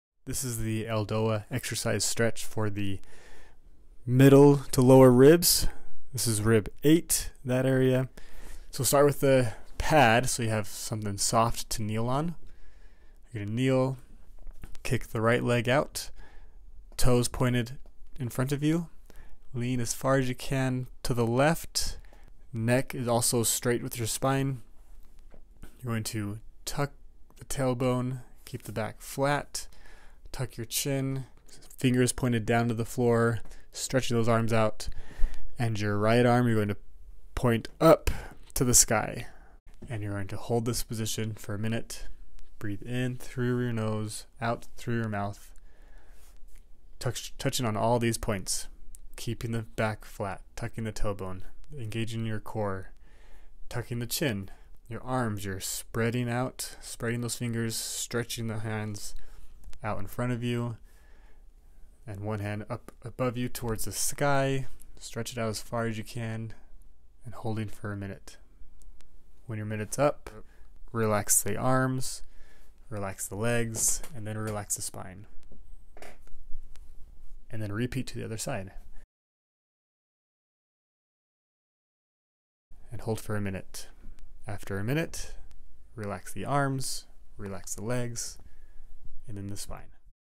ELDOA Rib 8 Stretch | P{rovo Chiropractor | Dr. Jared Wadsworth
Golf Yoga. Here is a fascial stretch for the middle-lower ribs.  Great to loosen some fascia/muscles and increase some function and mobility to the rib cage.
Get to help increase the range of motion to your rib face and increase the ability to bring the club back farther.
Located in Provo, Utah and helping golfers in the valley.